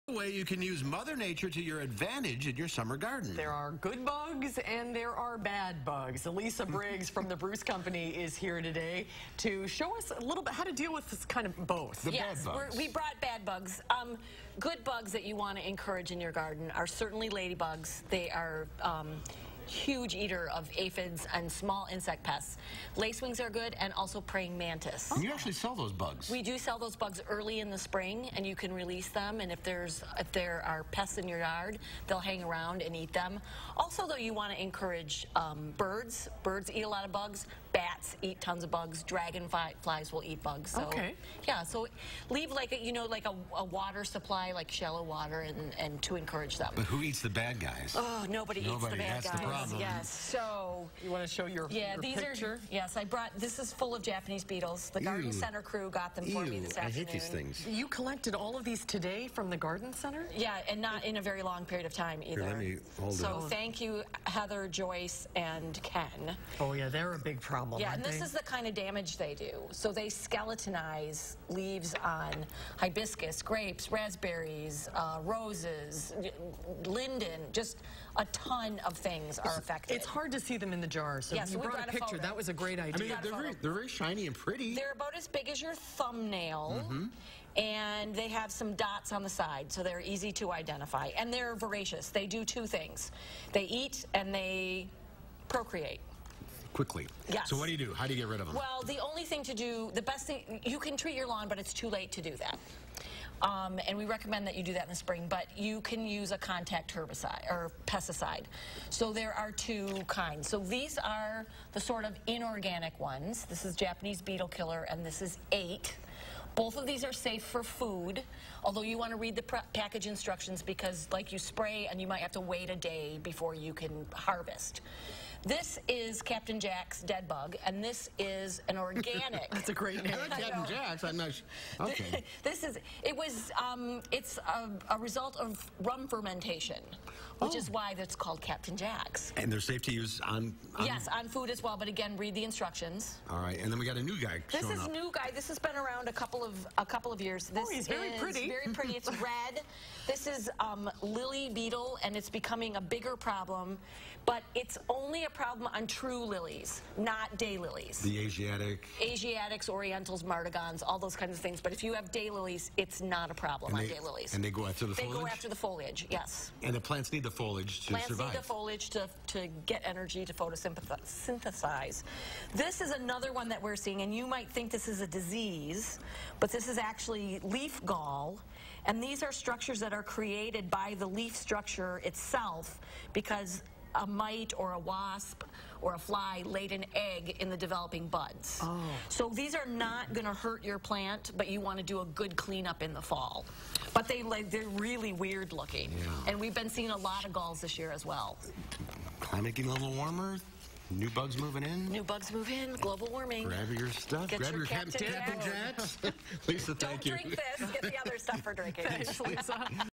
Beneficial insects for your garden
Lisa Briggs shares what bugs can help your garden.